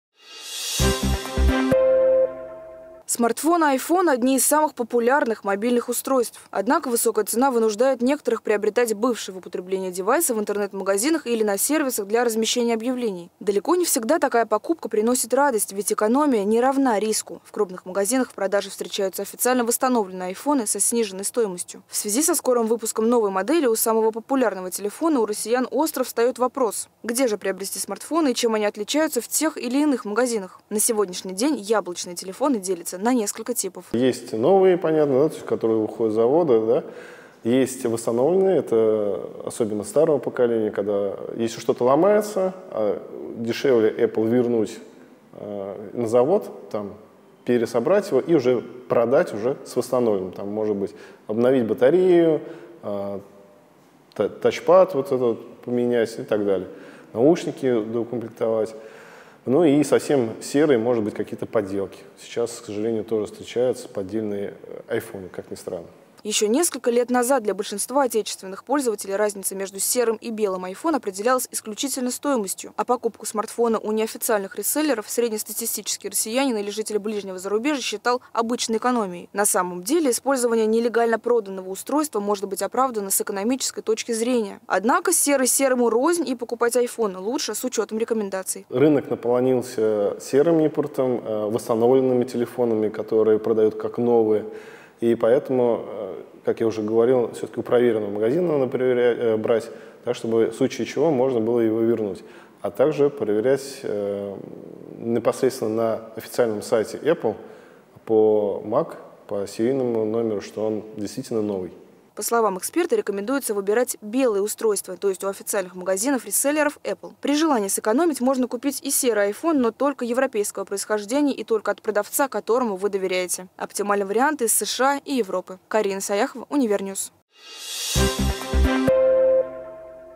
APPLE В КАЖДОМ ДОМЕ
Айфоны, безусловно, имиджевая вещь и требует осознанного подхода к покупке. О том, какие подводные камни есть на рынке смартфонов, смотрите далее.